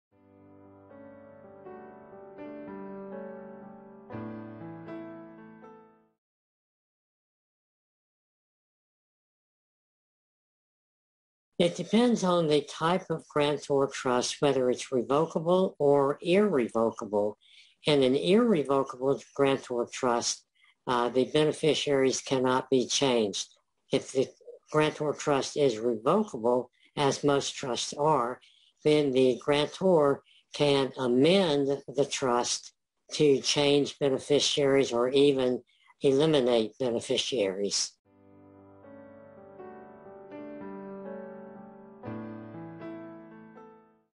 How Easily Beneficiaries On A Grantor Trust Can Be Modified | David Greene - SC
The Greene Law Firm, P.A.
11 McGee Street
Greenville, SC 29601
United States

The ease of adjusting various aspects of the trust depends on the type of grantor trust, whether it’s revocable or irrevocable. In an irrevocable grantor trust, the beneficiaries cannot be changed. However, if the grantor trust is revocable as most trusts are, the grantor can amend the trust to change beneficiaries or even eliminate them.